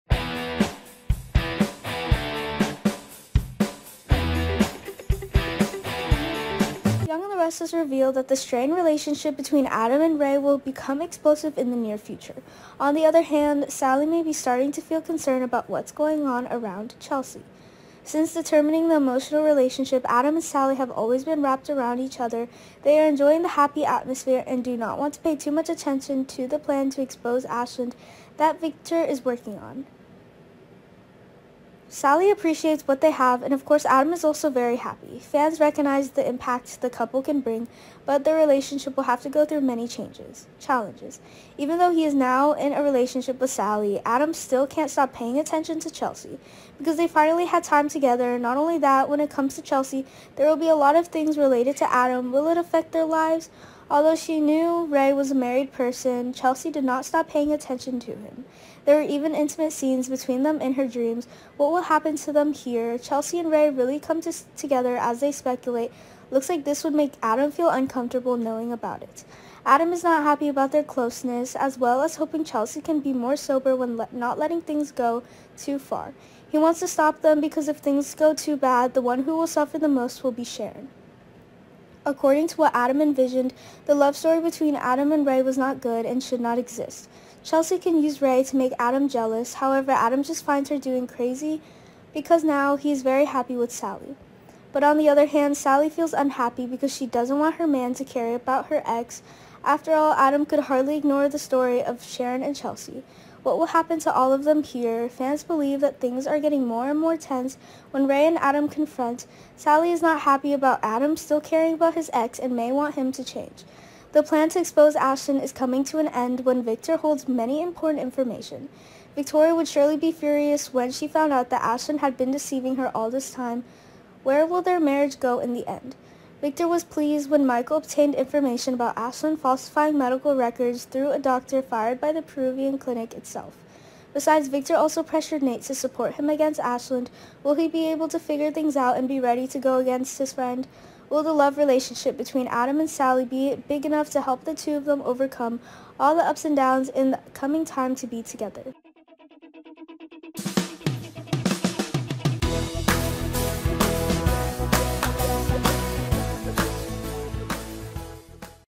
The Young And The Restless Spoilers All Victor's plans fail, Newman may go bankrupt
The Young And The Restless Spoilers Y&R All Victor's plans fail Newman may go bankrupt.